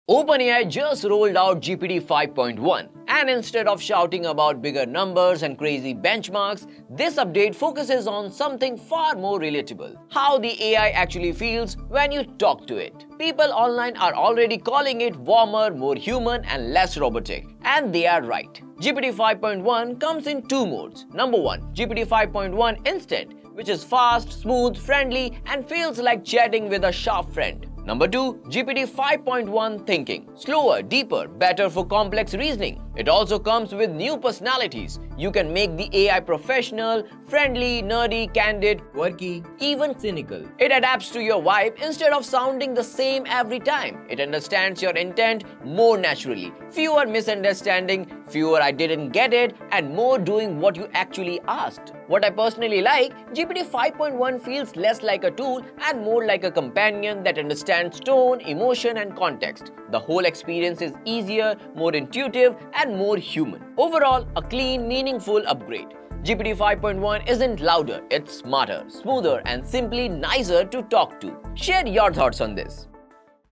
Is GPT-5.1 is maybe the Best GPT model yet?
OpenAI just launched GPT-5.1 with two new modes, human-like personalities, and natural conversational flow — making AI feel more like a companion than a chatbot.